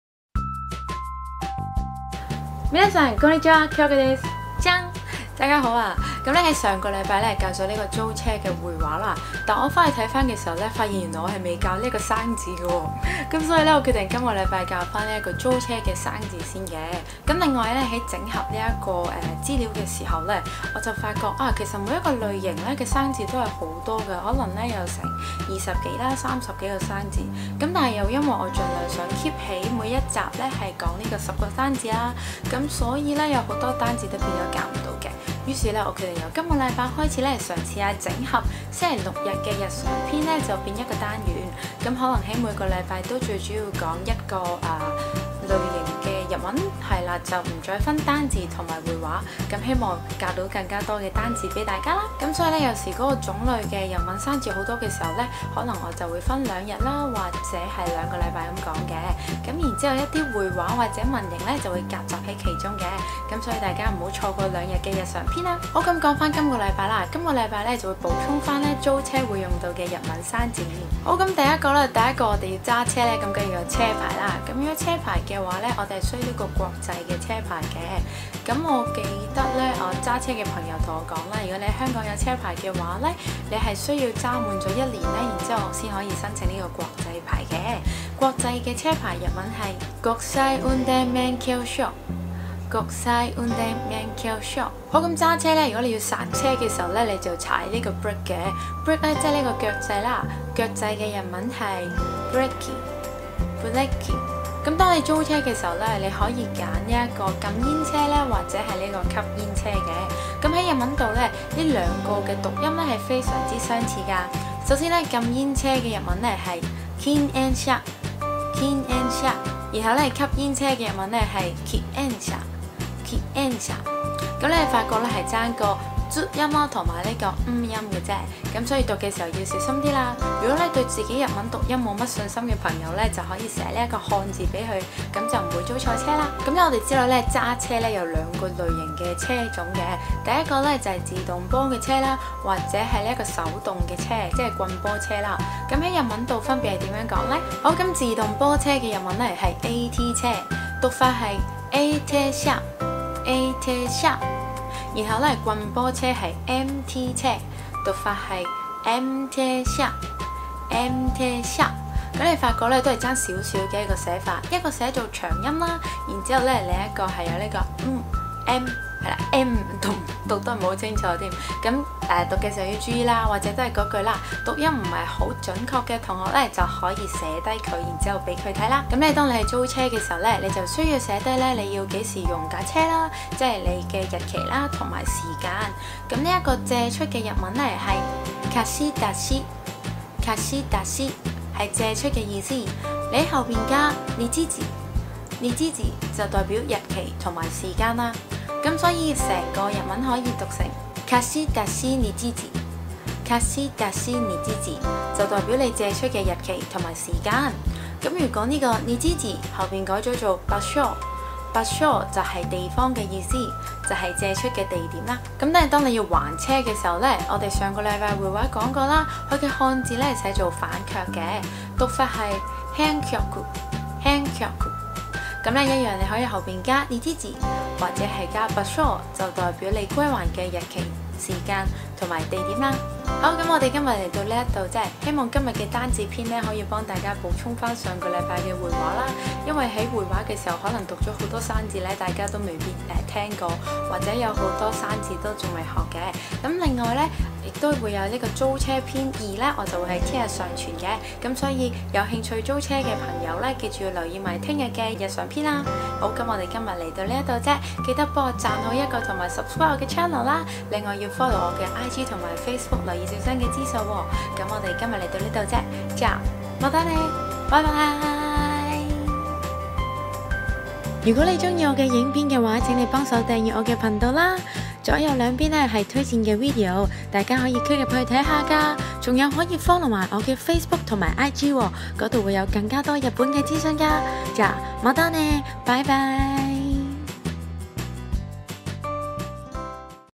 旅遊日語 租車 日文 日常篇: 上星期原來忘記分享租車的單字了~嘿嘿 #43 [黑貓響子]
上星期原來忘記分享租車的單字了!!
今個星期會補唷~~
另外整合了日常篇單字與會話不再分開了~~
希望分享更多的日文~~:)